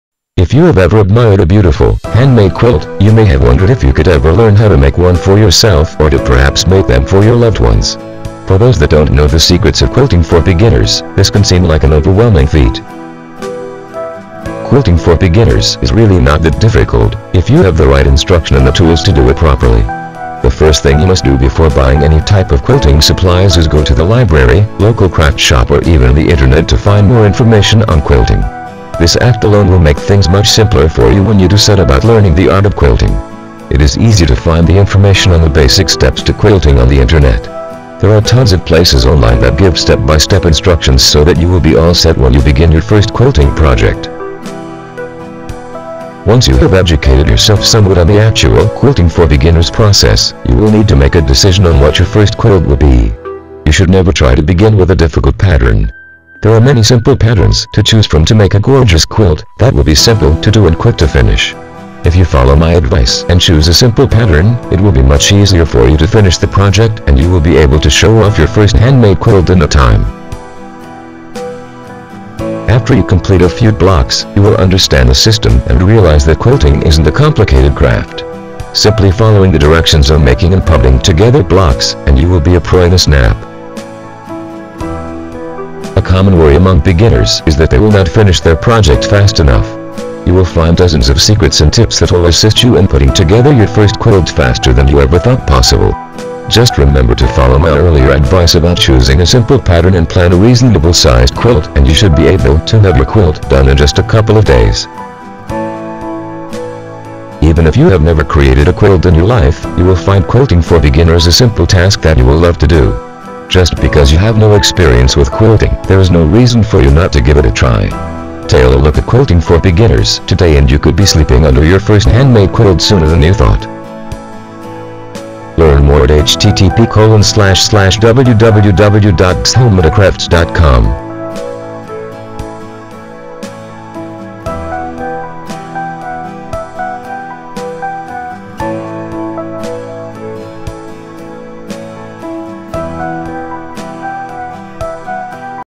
Quilting for Beginners - Yes! Even YOU Can Do It!
If you have ever admired a beautiful, handmade quilt, you may have wondered if you could ever learn how to make one for yourself or to perhaps make them for your loved ones.